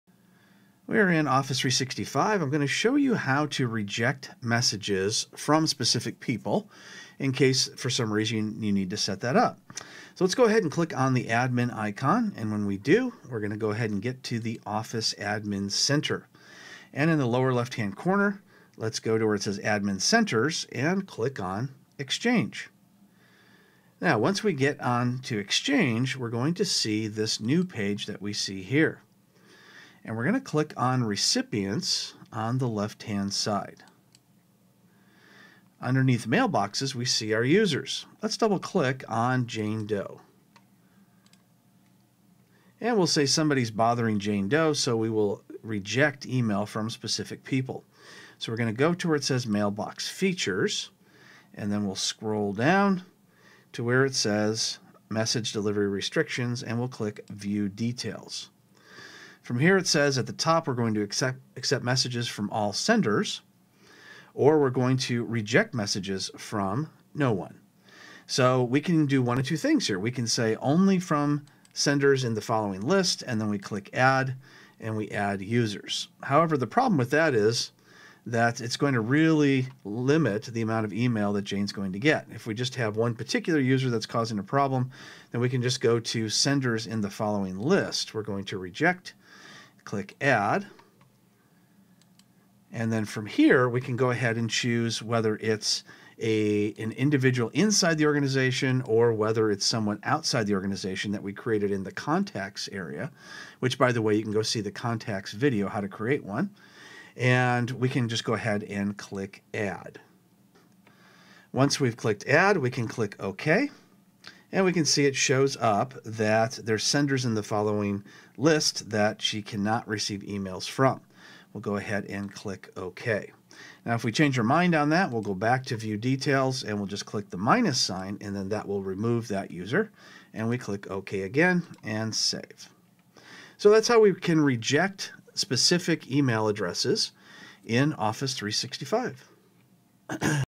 Microsoft 365 Tutorial How to reject email from specific users in Microsoft 365
Professor Robert McMillen shows you how to reject email from specific users in Microsoft 365. If you have a user being bothered by someone else sending them email, this allows you to block them or an entire domain from sending that person emails.